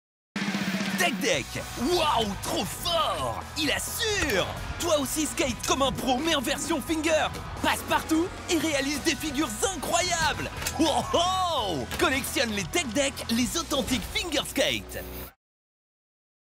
Tech Deck – les authentiques fingerskate
Tech Deck, les varies fingerskates avec des graphismes de skate sympas. Collectionne-les toutes et montre des figures de skate cool à tes amis. Quelles sont les figures de skate que tu sais déjà faire ?